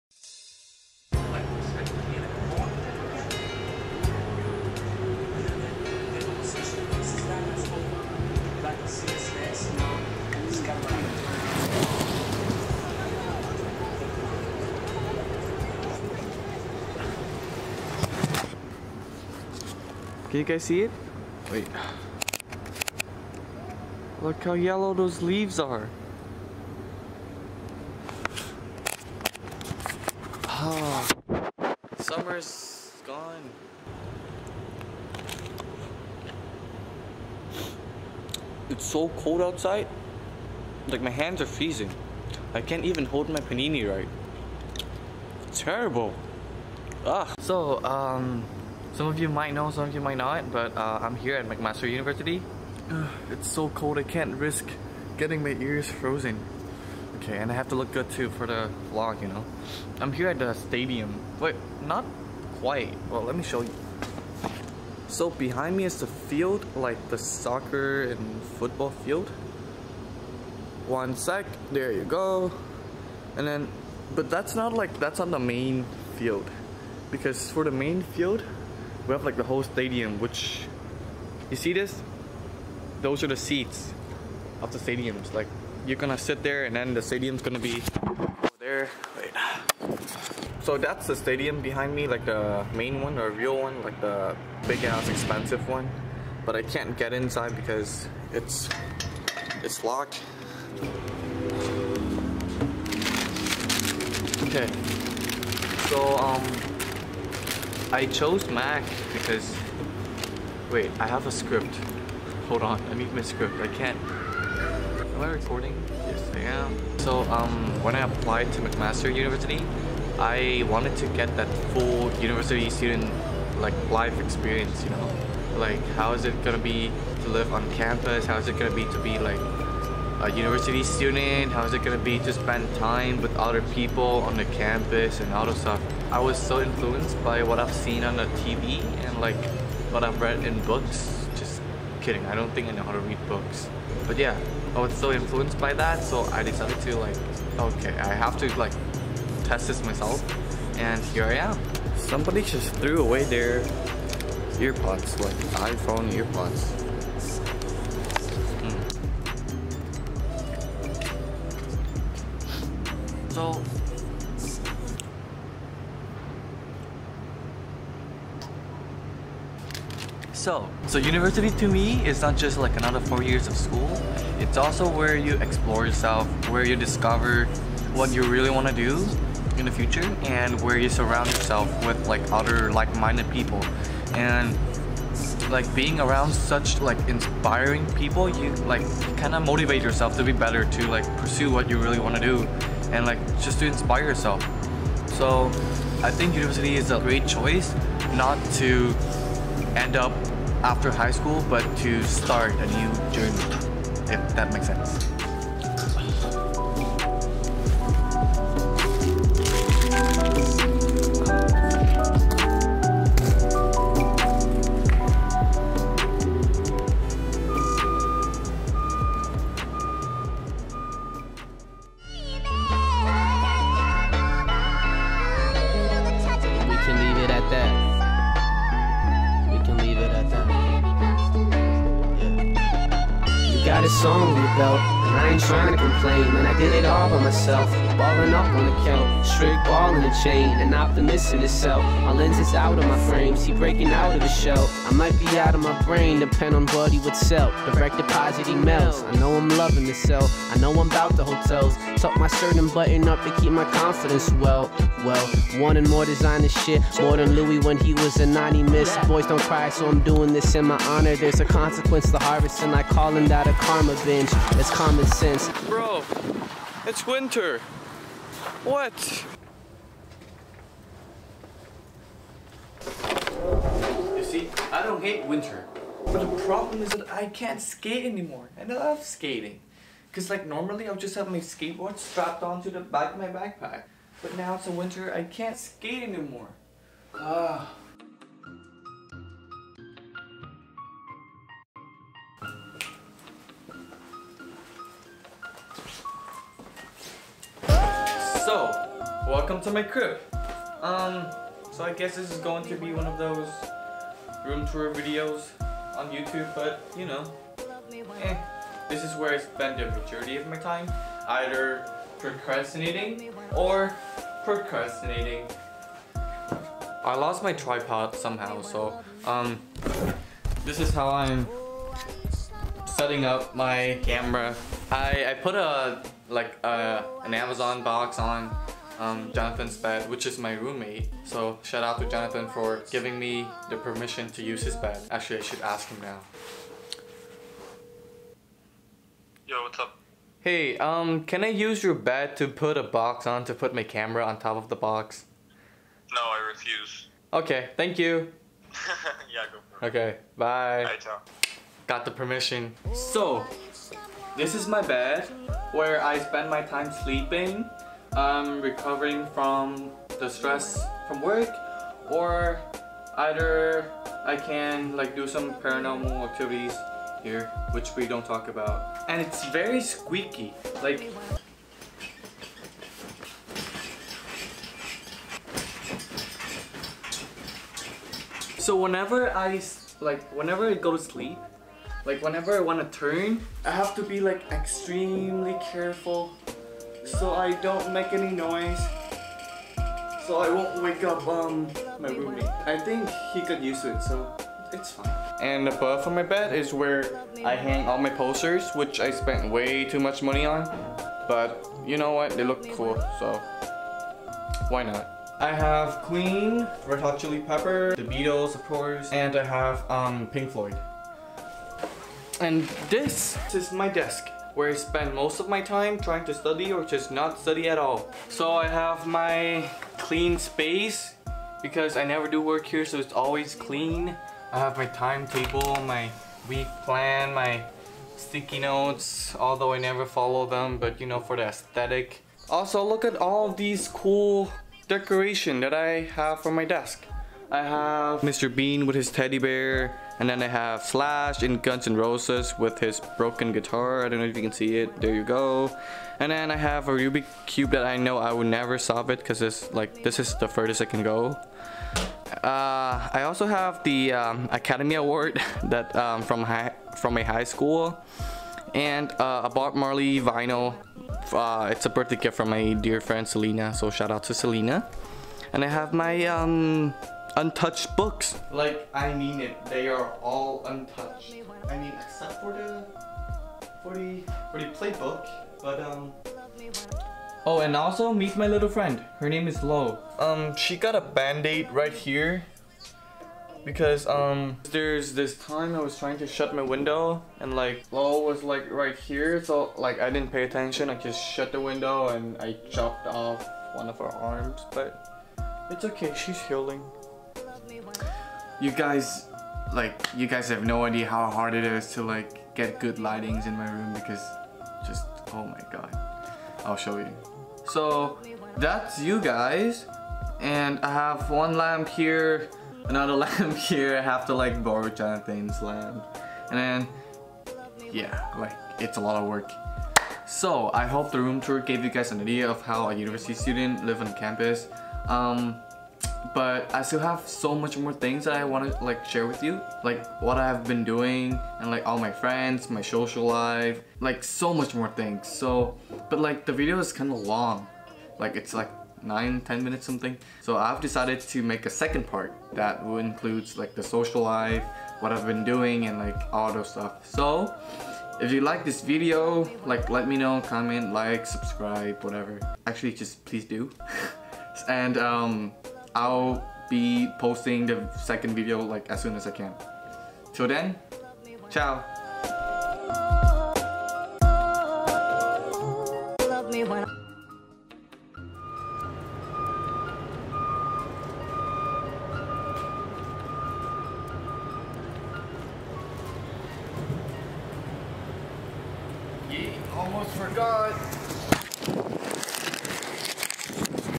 McMaster's Residence Room Tour (kinda) | Life in University
This little room tour is meant to give you a closer look at a typical university student's room and have an idea of what it is like to live on campus.
Let me know if you guys like the video and show your support! I'd really appreciate it since I literally spend a whole day not just to edit and missed out a bunch of work :( but you know what, it's worth it!